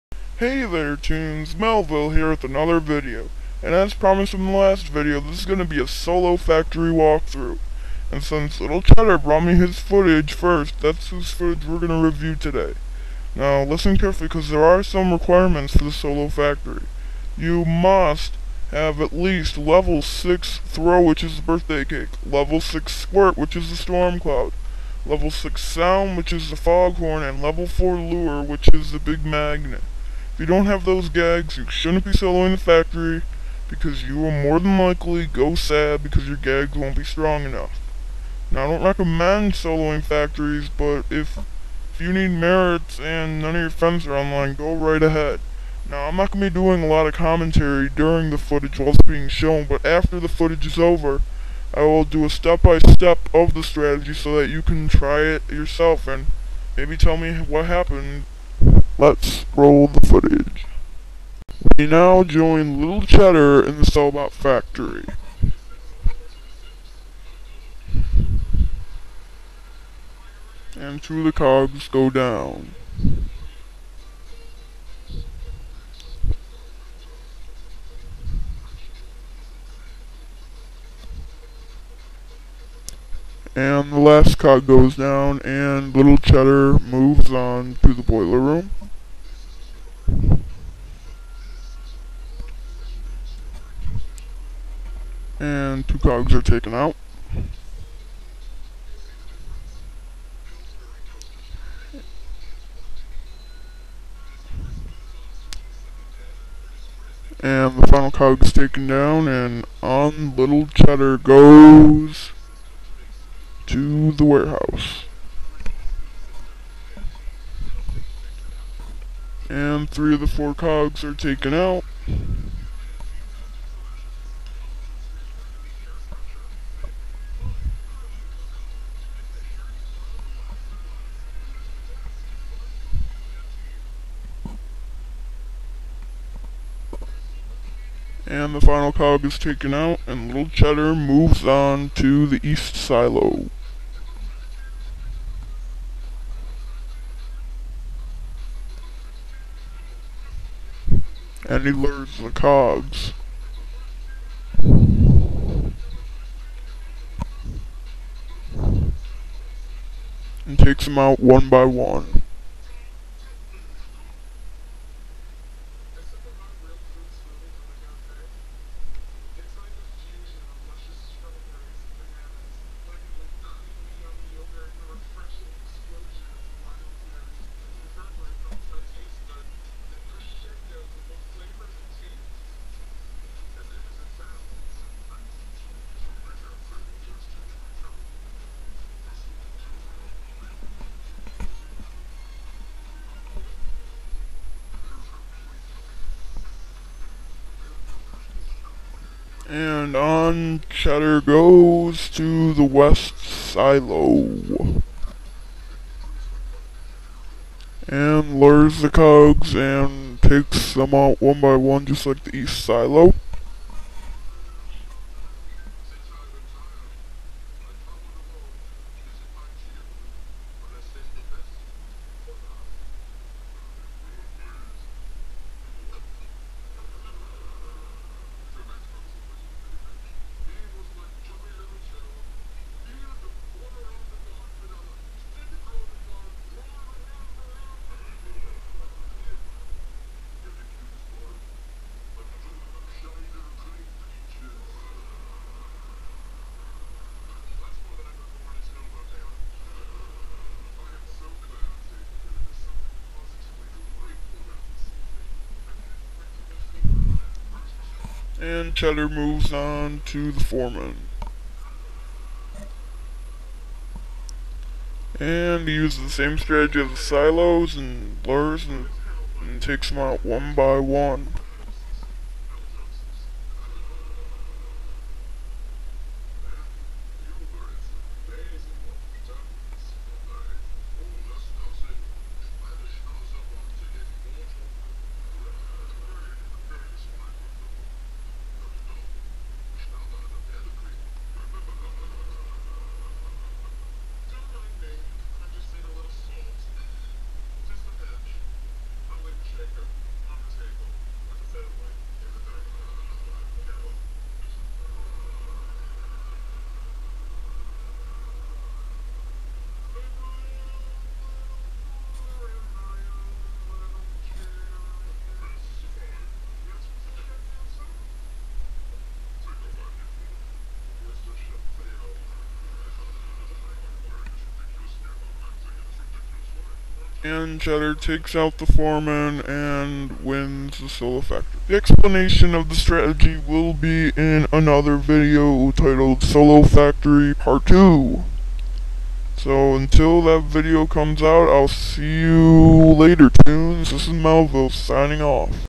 Melville -- Solo Factory Walkthrough Part 1
Melville reviews Little Cheddar's footage of a solo factory.